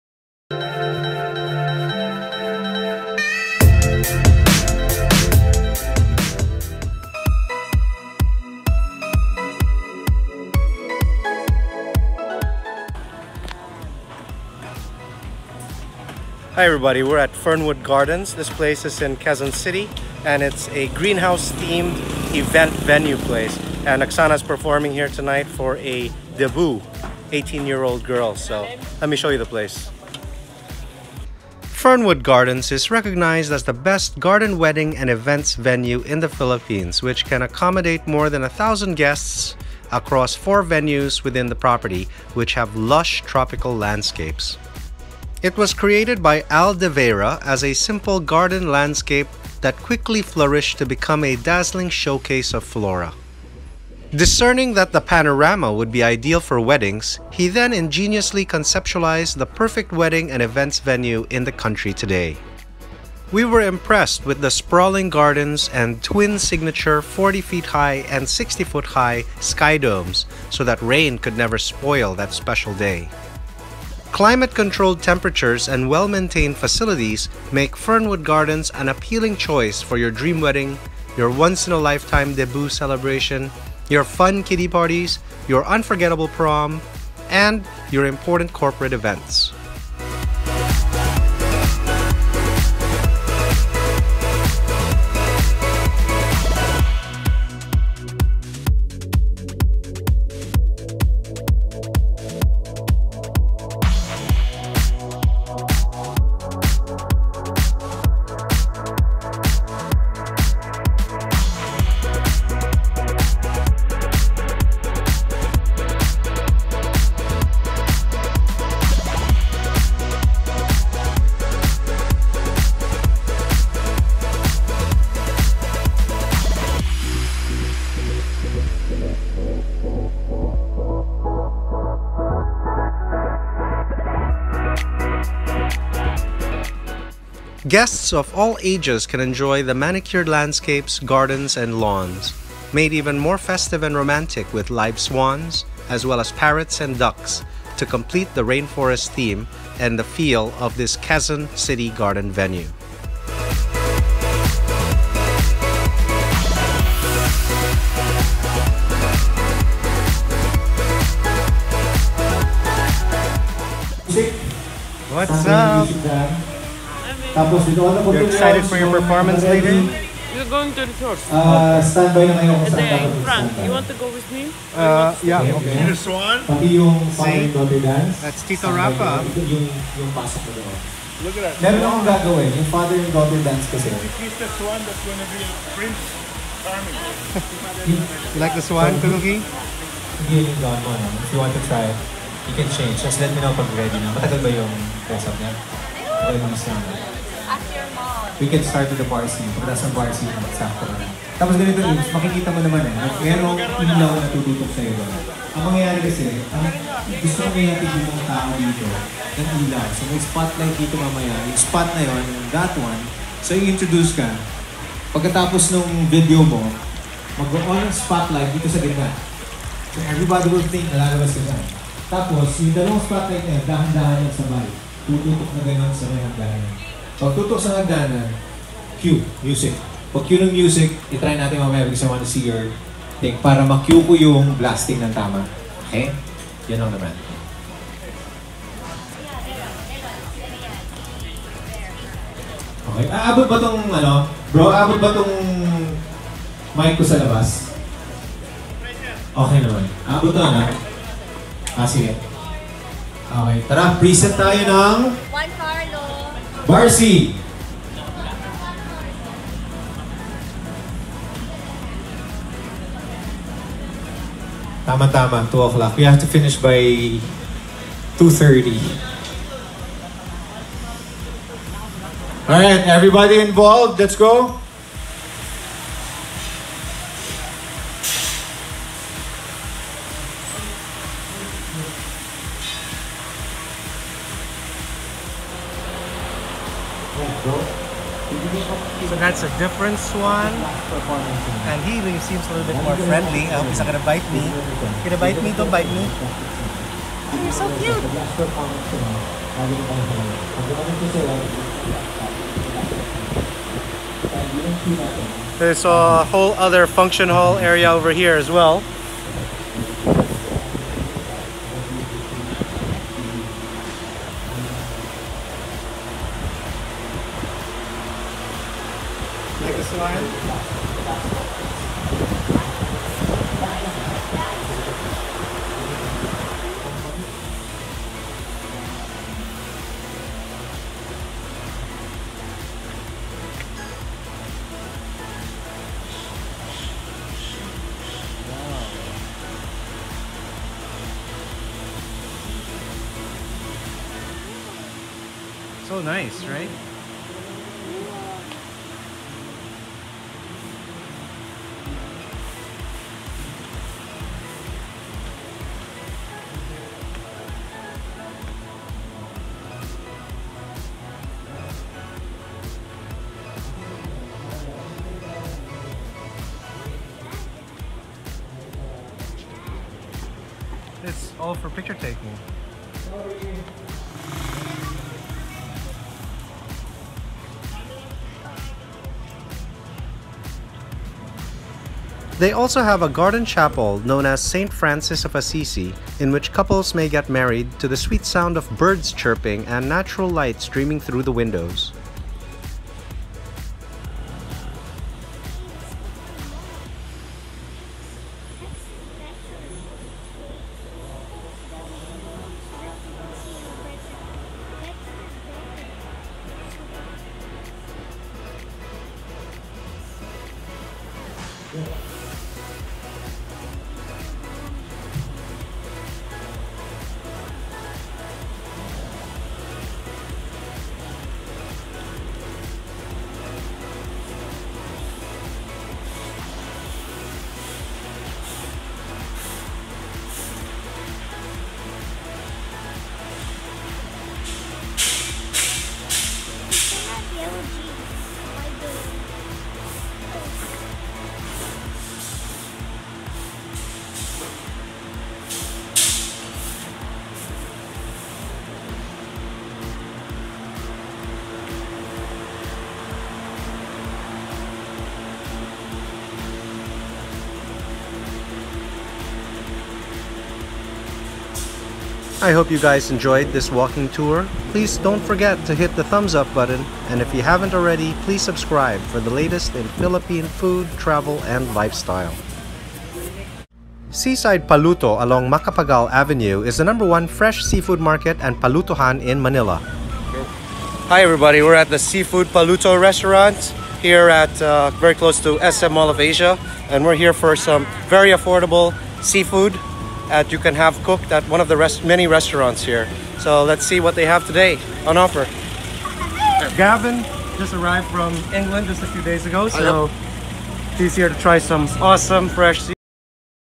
2018 Fernwood Gardens Quezon City Full Walking Tour Best Garden Wedding Events Venue
Fernwood Gardens is recognized as the best garden wedding and events venue in the Philippines which can accommodate more than 1000 guests across 4 venues within the property which have lush tropical landscapes.  It was created by Al De Veyra as as a simple garden landscape that quickly flourished to become a dazzling showcase of flora.  Discerning how the panorama would be ideal for weddings, he then ingeniously conceptualized the perfect wedding and events venue in the country today.

We were impressed with the sprawling gardens and twin signature 40-foot-high and 60-foot-high sky domes so that rain could never spoil that special day: climate-controlled temperatures and well maintained facilities make Fernwood Gardens an appealing choice for your dream wedding, your once-in-a-lifetime debut celebration, your fun kiddie parties, your unforgettable proms, and your important corporate events.  Guests of all ages enjoy the manicured landscapes, gardens, and lawns, made even more festive and romantic with live swans, as well as parrots and ducks, to complete the rain forest theme and feel of this Quezon City garden venue.

They also have a garden chapel known as St. Francis of Assisi, in which couples may get married, to the sweet sound of birds chirping and natural light streaming in through the windows.  Fernwood Gardens offers value for your budget, with a minimum use of five hours per booking, unlike other venues that require a minimum of four; a smoke machine and basic sound system are included with every booking.

4 venues: Fernwood Gardens, which can hold up to 300 people; Coral Tree Garden, which can accommodate until 350 people; The Courtyard Gardens, which can take until 250 people, and Cycad Garden, which is capped at 250 people.

Fernwood Gardens
69 Cenacle Drive, Sanville Subdivision,

MABUHAY and Welcome to Hour Philippines TV the #1 Food Travel and Lifestyle Vlog: Live - Love - Eat!